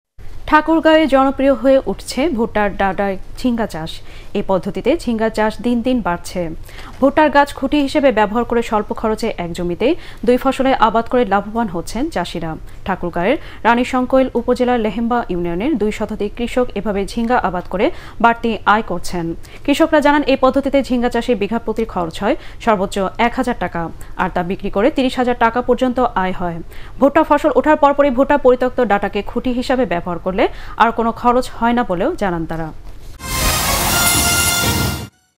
ঠাকুরগাঁওয়ে জনপ্রিয় হয়ে উঠছে ভুট্টার ডাটায় ঝিঙ্গা চাষ।
ঠাকুরগাঁওয়ে জনপ্রিয় হয়ে উঠছে ভুট্টার ডাটায় ঝিঙ্গা চাষ। এই পদ্ধতিতে ঝিঙ্গার চাষ দিন দিন বাড়ছে। ভুট্টার গাছ খুঁটি হিসেবে ব্যবহার করে স্বল্প খরচে এক জমিতেই দুই ফসলের আবাদ করে লাভবান হচ্ছেন চাষিরা। ঠাকুরগাঁওয়ের রাণীশংকৈল উপজেলার লেহেম্বা ইউনিয়নের দুই শতাধিক কৃষক এভাবে ঝিঙ্গা আবাদ করে বাড়তি আয় করছেন। কৃষকরা জানান, এই পদ্ধতিতে ঝিঙ্গা চাষে বিঘা প্রতি খরচ হয় সর্বোচ্চ এক হাজার টাকা। আর তা বিক্রি করে ৩০ হাজার টাকা পর্যন্ত আয় হয়। ভুট্টা ফসল উঠার পর পরই ভুট্টার পরিত্যক্ত ডাটাকে খুঁটি হিসাবে ব্যবহার করলে আর কোন খরচ হয় না বলেও জানান তাঁরা।